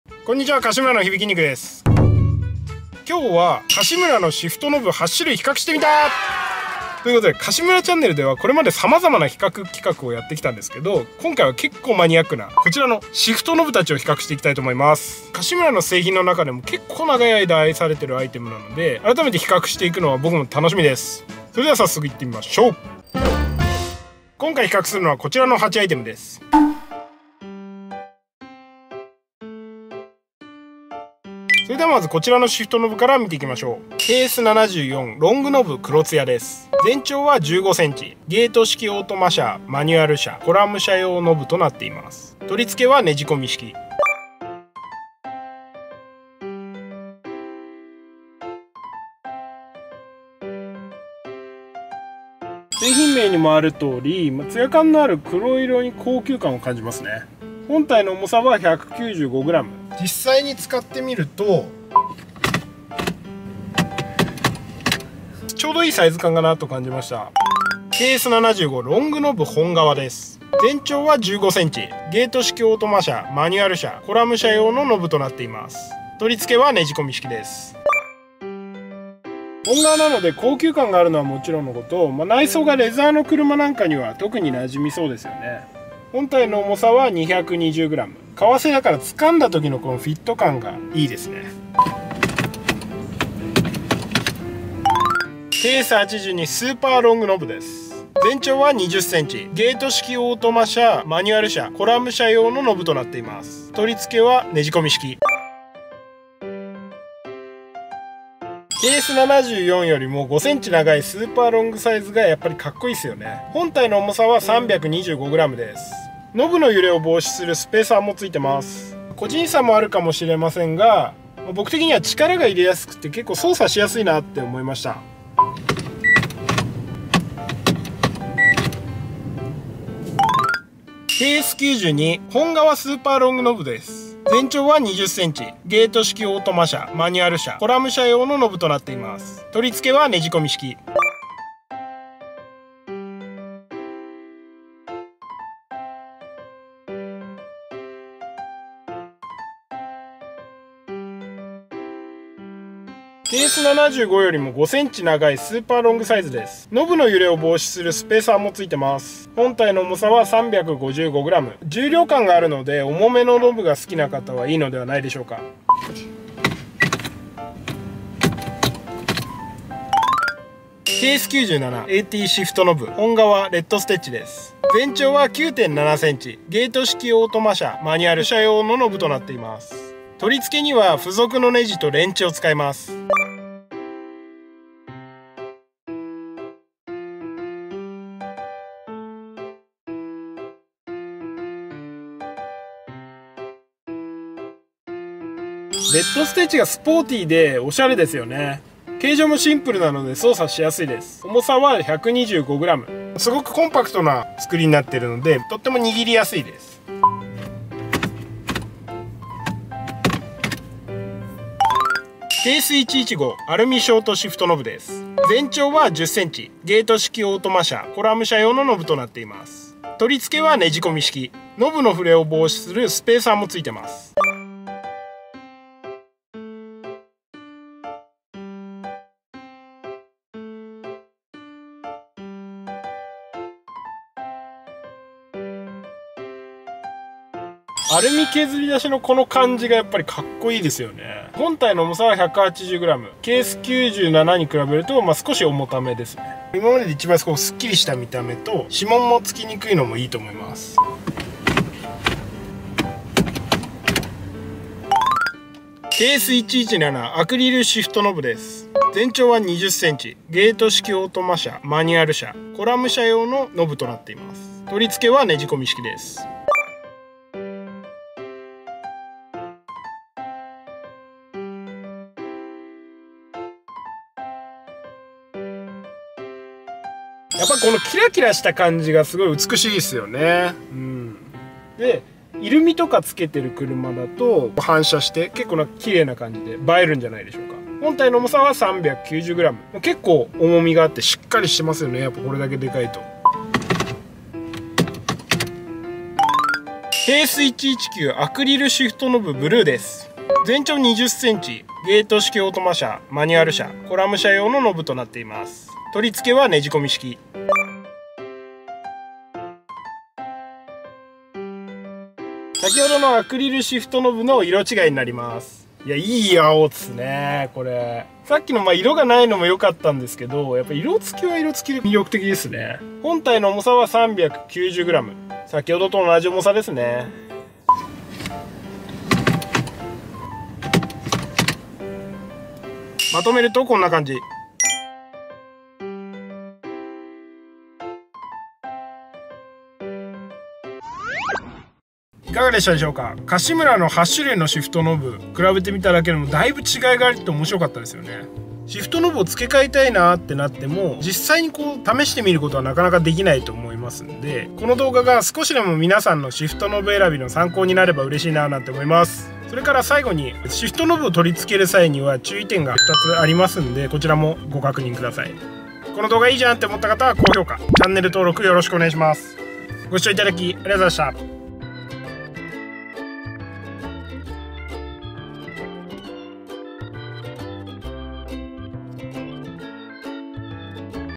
【かっこいい】カシムラのシフトノブ8種類徹底比較！
こんにちは。
カシムラチャンネルです。

今回は、カシムラのシフトノブ8種類を徹底比較してみました。
ちょっとマニアックな今回のテーマですが、カシムラ製品の中でもシフトノブは長く売れ続けている製品なんですよね。
皆さんはどんなタイプのシフトノブがお気に入りでしょうか？
ちなみにHIBI★きんにくはアクリルシフトノブが断然使いやすいと言っていました。